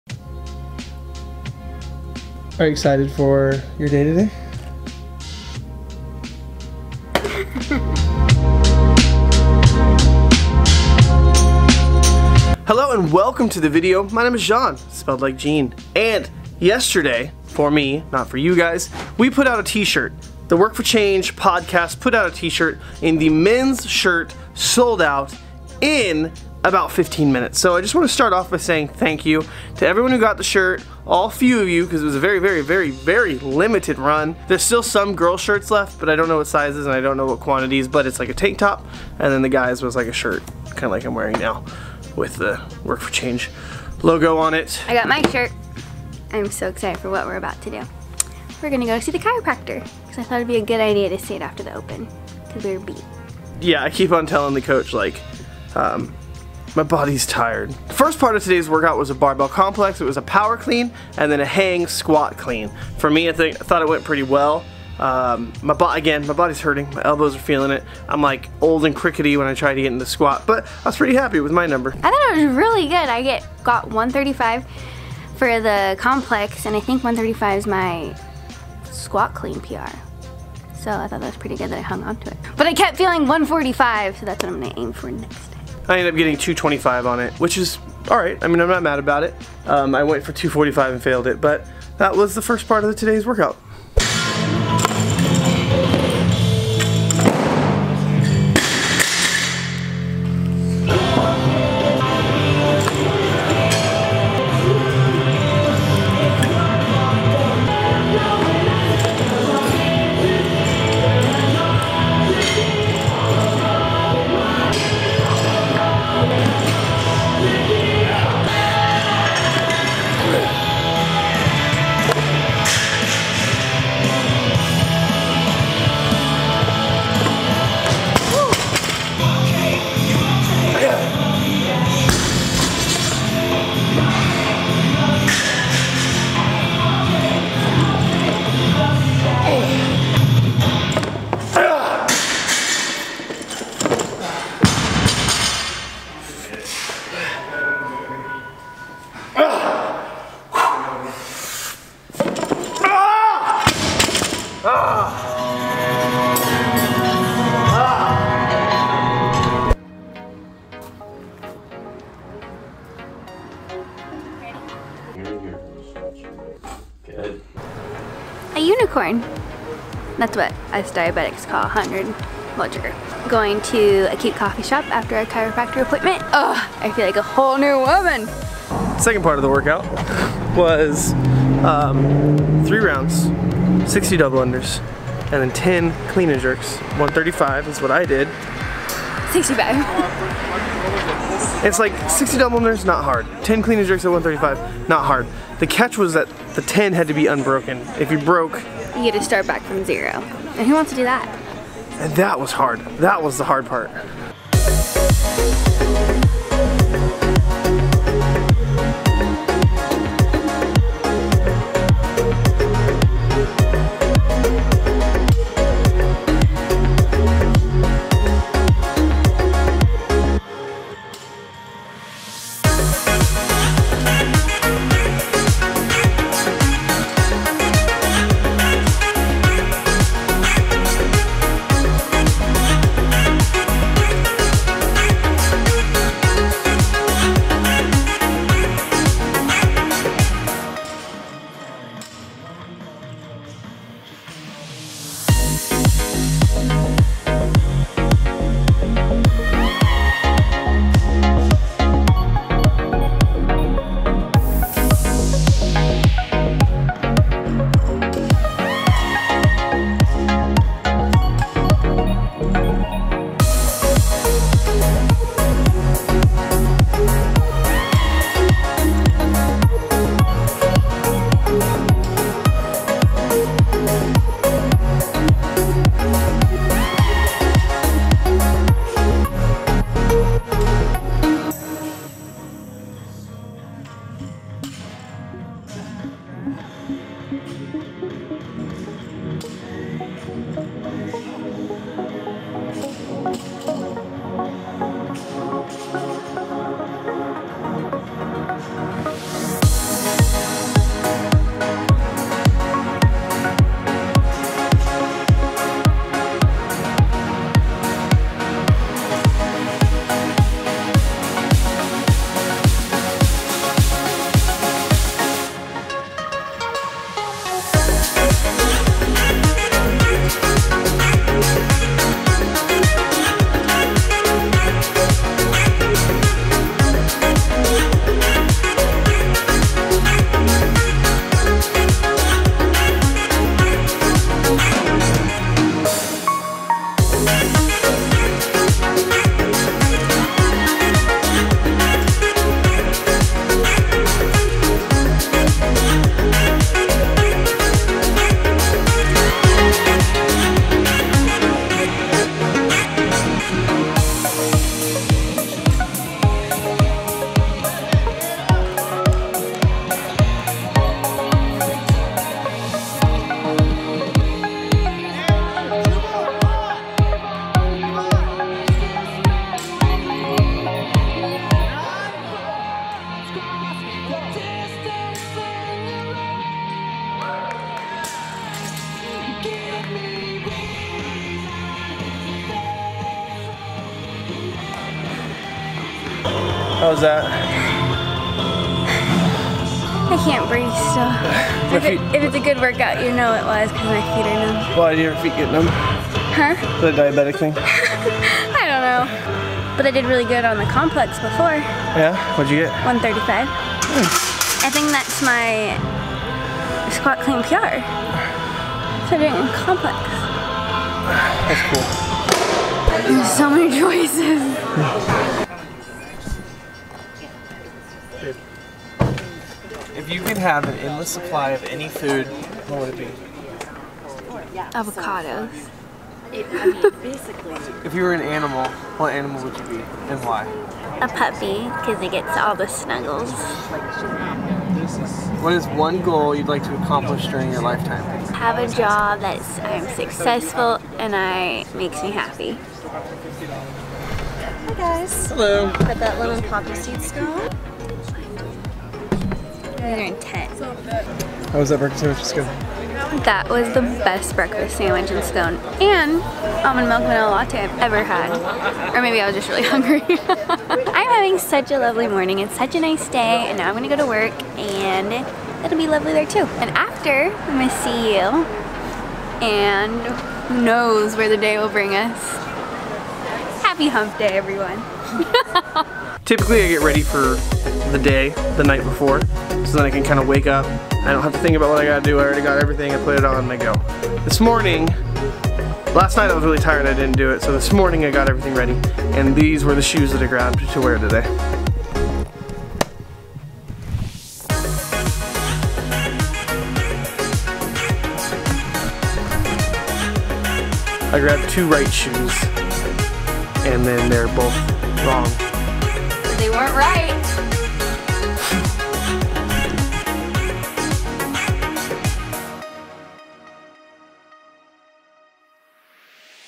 Super Grippy CrossFit Workout | CrossFit + Lifestyle Vlog
Instagram: @jeanglaude @samhale10 Twitter: @thejeanglaude

Today's Workout was:
15 Min EMOM:
1 Power Clean
1 Hang Squat Clean

Metcon:
3 Rounds for Time
60 Double Unders
10 Clean and Jerks (95/65) or (135/95)
C+J Must be unbroken

8 week courses to improve specific skills (link saves you $$)

CrossFit - Vlog - Blog - Fitness - Health